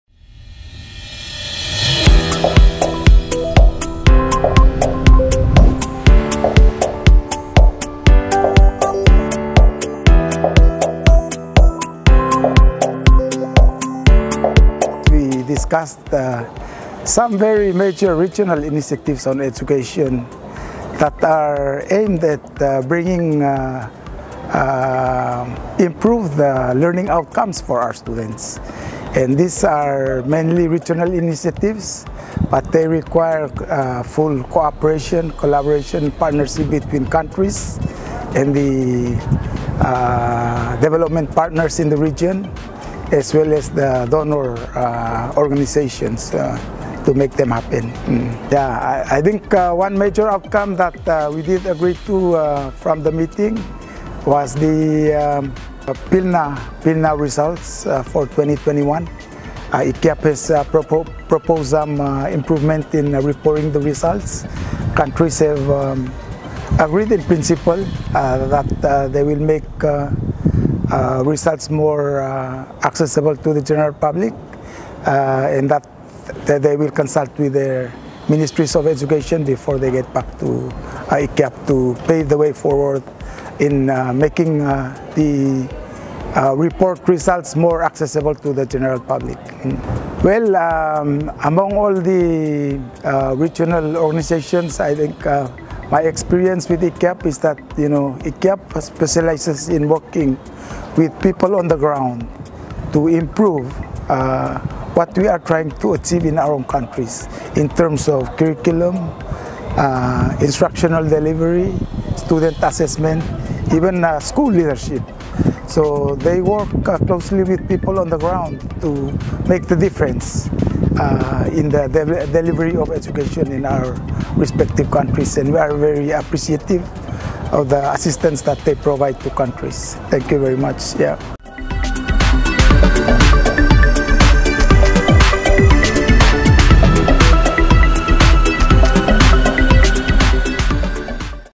A recap of PBEQ 2023: Palau
Meet Raynold Mechol, Palau’s Senior Education Officer and the Chair of the Pacific Board for Education Quality (PBEQ) meeting 2023, providing a recap of the outcomes of last year's PBEQ meeting and highlighting the role of SPC in fulfilling the education needs of the Pacific countries.

The PBEQ, a subcommittee of the Committee of Representatives of Governments and Administrations (CRGA), governs SPC's work plans and programmes through annual convenings. The PBEQ convening provides an opportunity for the SPC team to engage in dialogues with its members and to understand their education priorities and needs. The inputs received from the members generate the future work plans of the SPC team.

The 2024 PBEQ meeting will be held in Nadi, Fiji from 20 to 22 March.